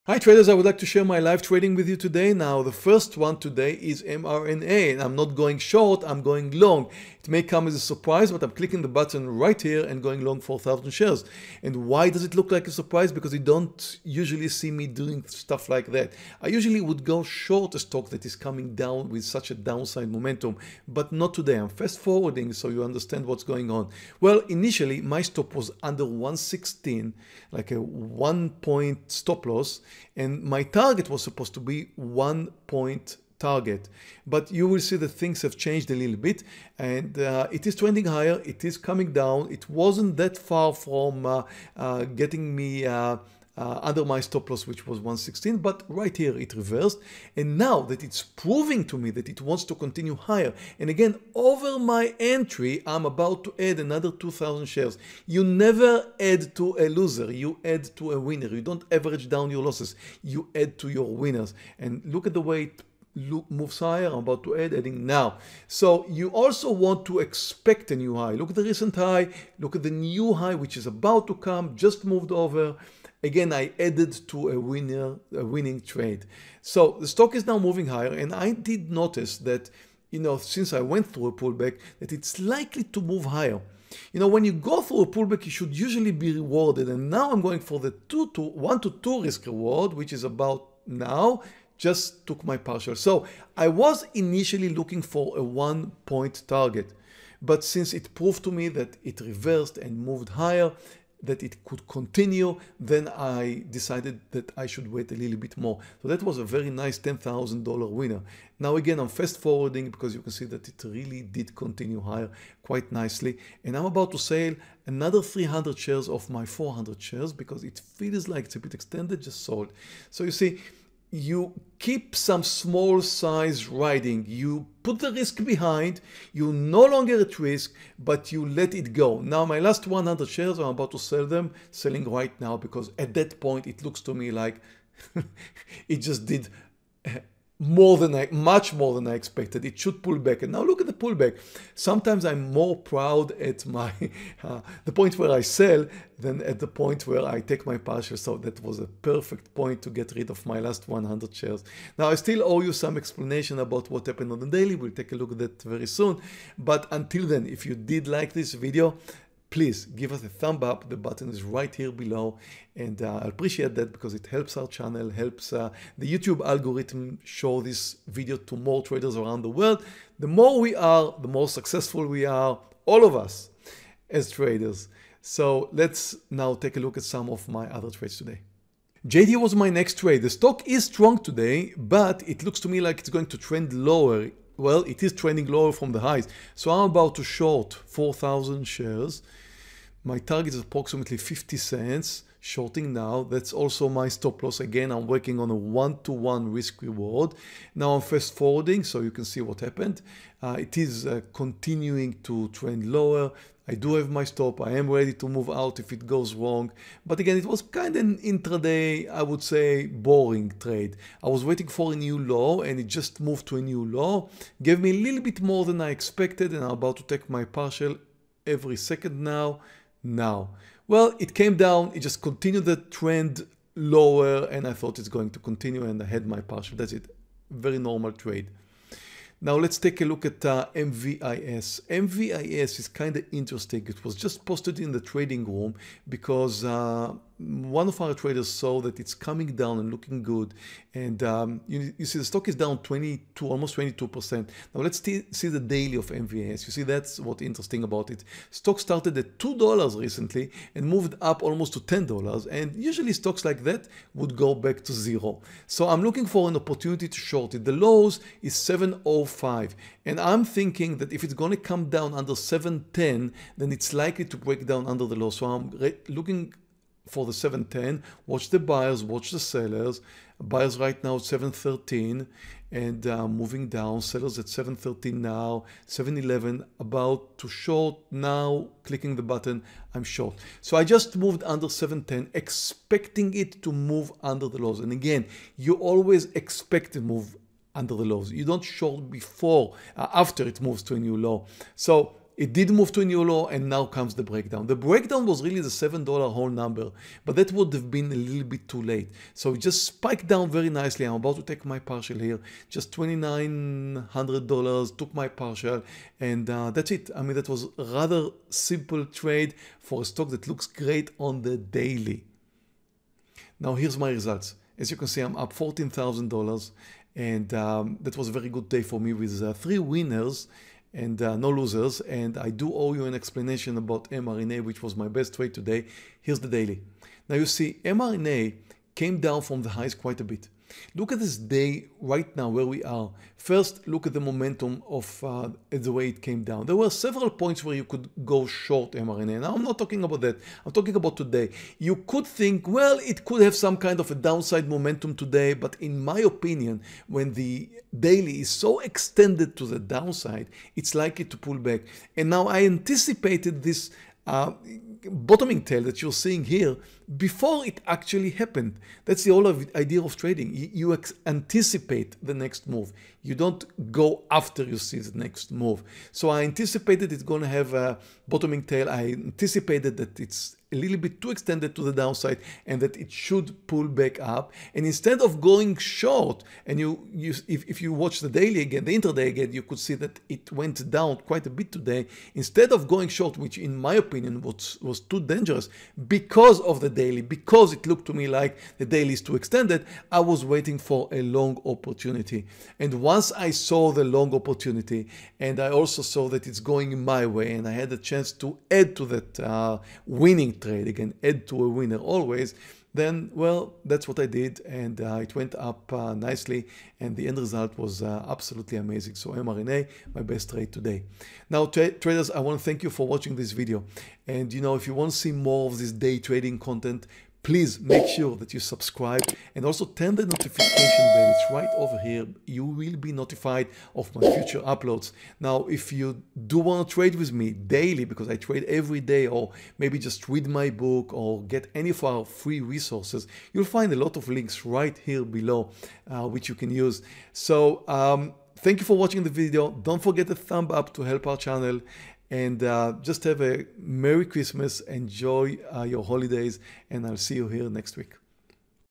$14,000 TRADING PROFIT
🎁 Special holiday prices across all of Tradenet's programs!

Live Day Trading Stocks - Earning a $14,000 profit trading MVIS, JD and MRNA. Meir Barak live day trading on December 23rd 2020.